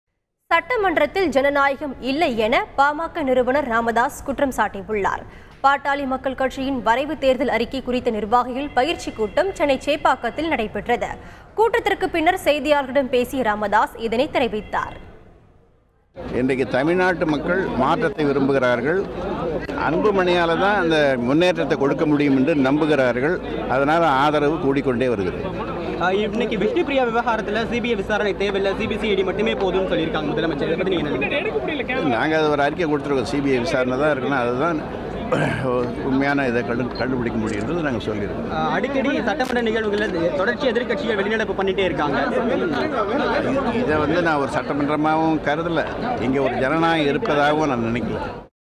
"No Democracy in Tamil Nadu Assembly" - Dr.S. Ramadoss : Thanthi TV
In Chennai, PMK Founder Dr.S.Ramadoss Says "No Democracy in Tamil Nadu Assembly" : Thanthi TV